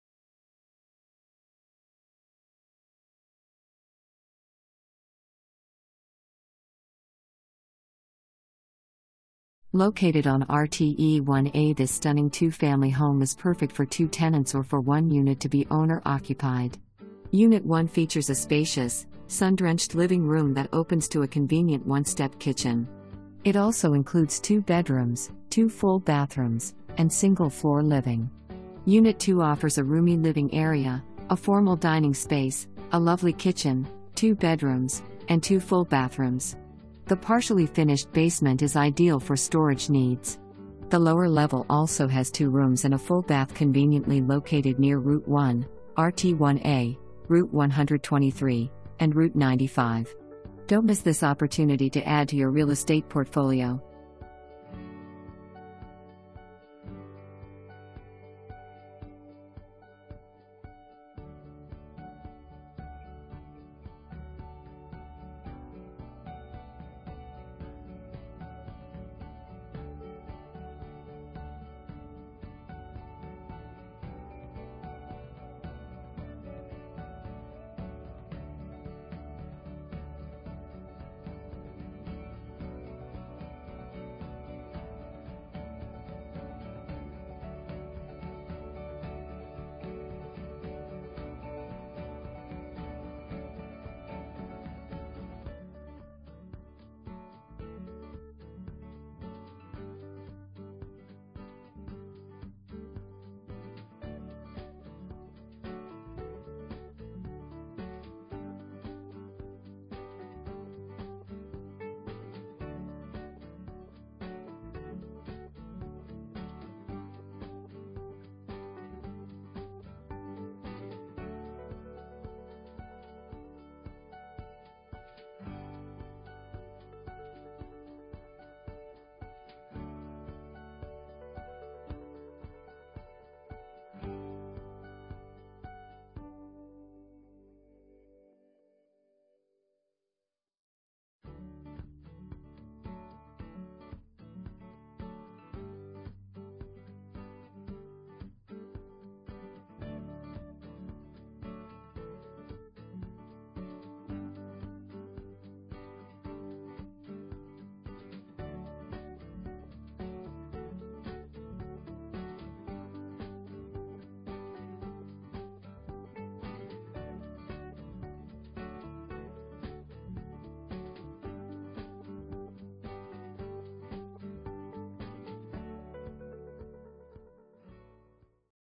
Homes for Sale - 782 Newport Ave, Attleboro, MA
782 Newport Ave
Attleboro, MA 02703

LOCATED ON RTE 1-A This stunning two-family home is perfect for two tenants or for one unit to be owner-occupied. Unit 1 features a spacious, sun-drenched living room that opens to a convenient one-step kitchen. It also includes 2 bedrooms, 2 full bathrooms, and single-floor living. Unit 2 offers a roomy living area, a formal dining space, a lovely kitchen, 2 bedrooms, and 2 full bathrooms. The partially finished basement is ideal for storage needs. The lower level also has 2 rooms and a full bath Conveniently located near Rt 1, Rt 1A, Rt 123, and Rt 95! Don't miss this opportunity to add to your real estate portfolio!

Bette Dixon
Keller Williams Realty